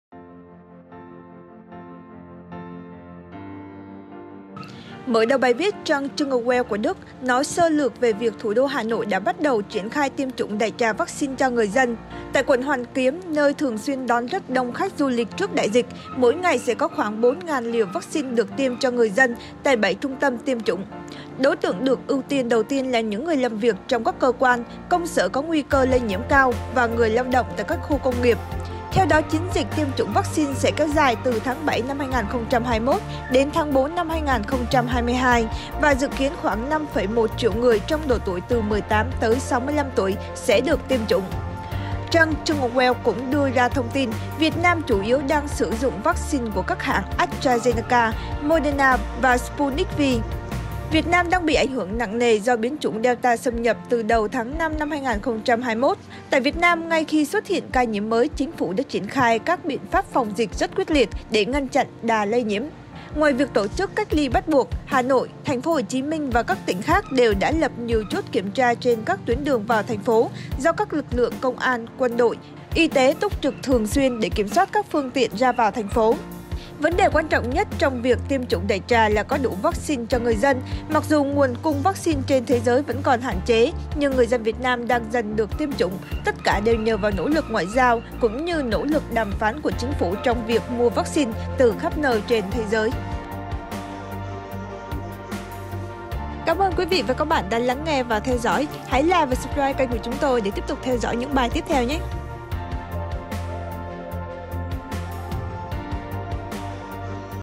Báo Đức Jungewelt: Nỗ lực tuyệt vời của Việt Nam trước đại dịch Covid 19
CanhCo Trang Jungewelt của Đức vừa có bài viết nói về nỗ lực tiêm chủng trên diện rộng của Việt Nam trong bối cảnh biến chủng Delta đang hoành hành trên diện rộng.

Mở đầu bài viết, trang Jungewelt nói sơ lược về việc Thủ đô Hà Nội đã bắt đầu triển khai tiêm chủng đại trà vaccine cho người.

Tại quận Hoàn Kiếm, nơi thường xuyên đón rất đông khách du lịch trước đại dịch, mỗi ngày sẽ có khoảng 4.000 liều vaccine được tiêm cho người dân tại 7 trung tâm tiêm chủng. Đối tượng được ưu tiên đầu tiên là những người làm việc trong các cơ quan, công sở có nguy cơ lây nhiễm cao và người lao động tại các khu công nghiệp, trang Jungwelt viết.

Theo đó, chiến dịch tiêm chủng vaccine sẽ kéo dài từ tháng 7/2021 đến tháng 4/2022 và dự kiến khoảng 5,1 triệu người trong độ tuổi từ 18 – 65 tuổi sẽ được tiêm chủng. Trang Jungwelt cũng đưa thông tin Việt Nam chủ yếu đang sử dụng vaccine của các hãng AstraZeneca, Moderna và Sputnik V.

Theo Jungwelt, Việt Nam đang bị ảnh hưởng nặng nề do biến chủng Delta xâm nhập vào từ đầu tháng 5/2021. Tại Việt Nam, ngay khi xuất hiện ca nhiễm mới, Chính phủ đã triển khai các biện pháp phòng dịch rất quyết liệt để ngăn chặn đà lây nhiễm.

Ngoài việc tổ chức cách ly bắt buộc, Hà Nội, Thành phố Hồ Chí Minh và các tỉnh khác đều đã lập nhiều chốt kiểm tra trên các tuyến đường vào thành phố do các lực lượng công an, quân đội, y tế túc trực thường xuyên để kiểm soát các phương tiện ra vào thành phố.

“Việc tiêm chủng cấp tốc cho toàn dân là nỗ lực rất lớn của Việt Nam để ngăn chặn dịch bệnh. Đến hiện tại, Việt Nam đã làm rất tốt điều này!”, trang Jungwelt nhận định.

Theo Jungwelt, vấn đề quan trọng nhất trong việc tiêm chủng đại trà là có đủ vaccine cho người dân. Mặc dù nguồn cung vaccine trên thế giới vẫn còn hạn chế, nhưng người dân Việt Nam đang dần được tiêm chủng, tất cả đều nhờ nỗ lực ngoại giao cũng như nỗ lực đàm phán của Chính phủ trong việc mua vaccine từ khắp nơi trên thế giới.

Bảo Trâm (Theo Jungwelt)

Cập nhật tin tức mới nhất